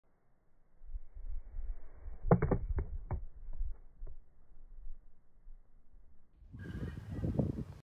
Croquet close strike 6 – FOUL!
This looks entirely wrong. The black ball hits the red, bounces back and hits the mallet again. Clearly a foul shot. But impossible to see without slow motion video.